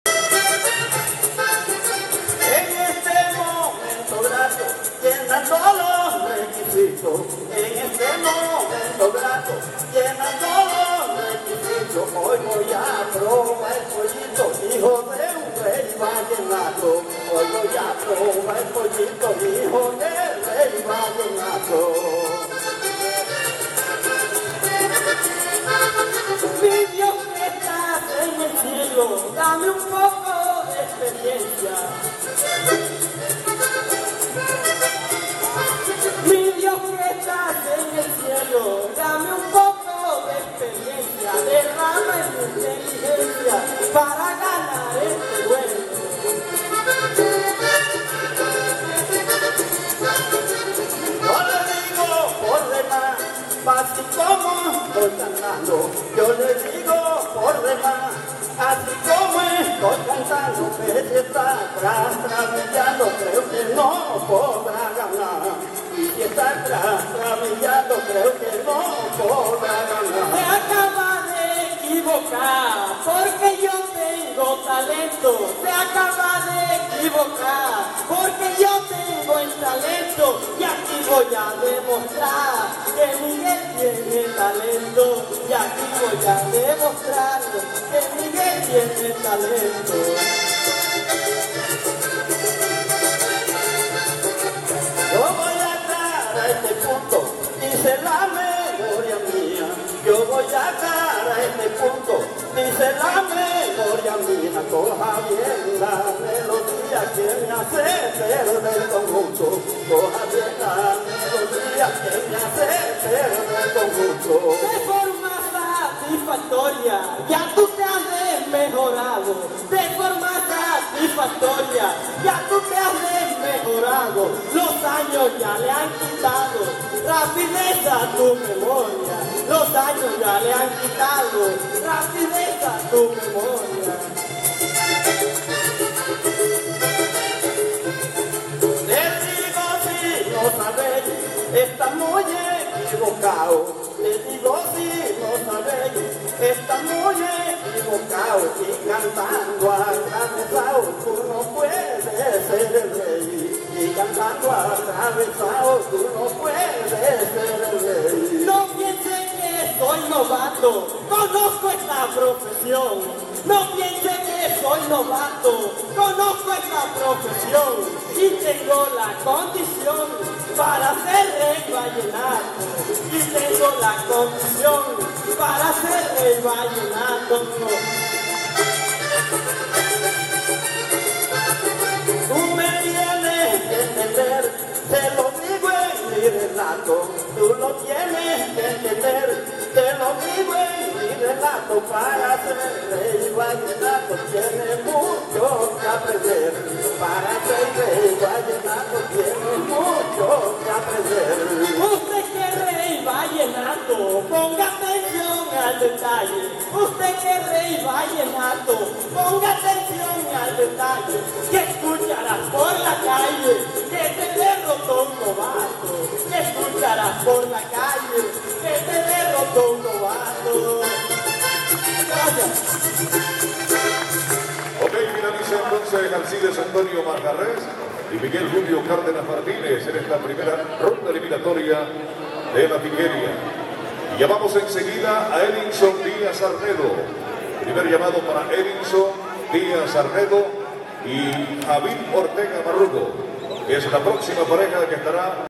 Piqueria Vallenata, Alcides Manjarrez VS Miguel Julio Cardenas
Primera ronda de piqueria, festival de la leyenda vallenata 2018